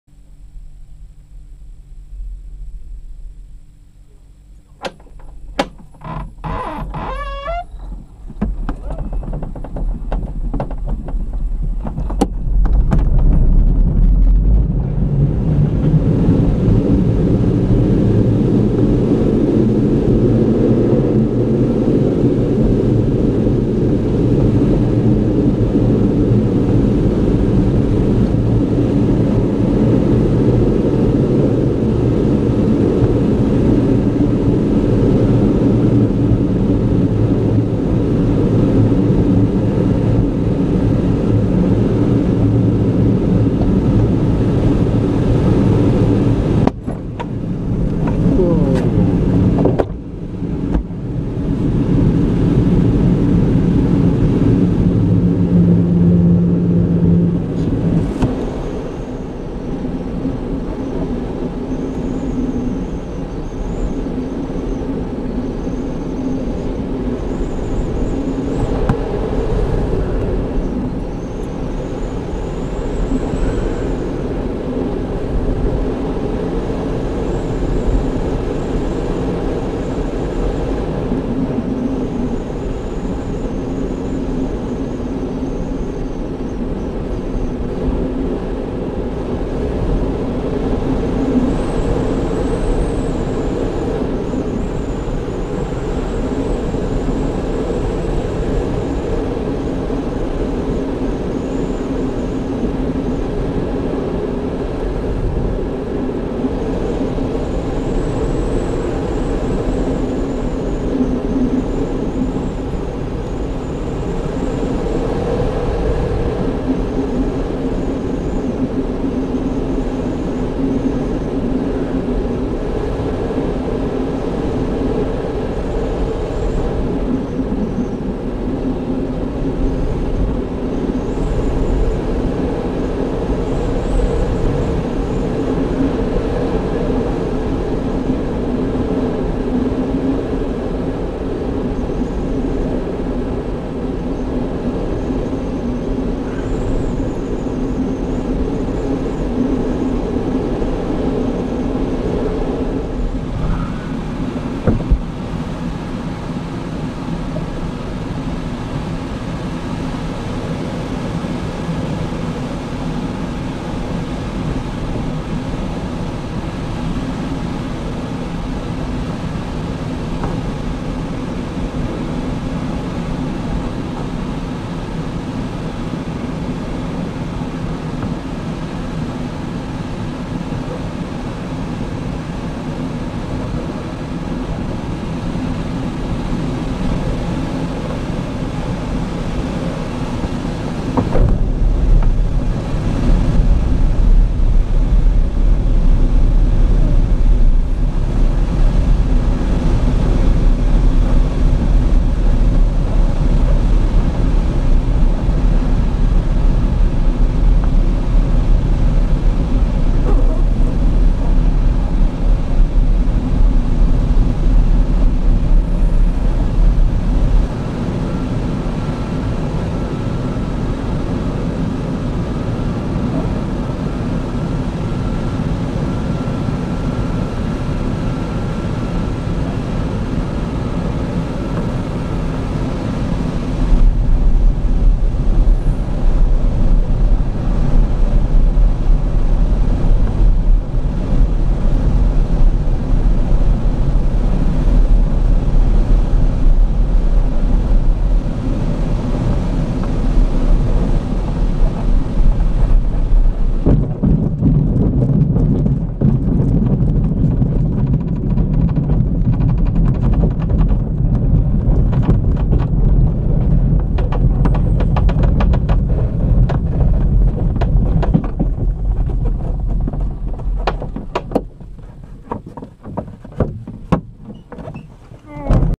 Glider Winch launch in my Libelle
Winch launch, some soaring and base leg/landing.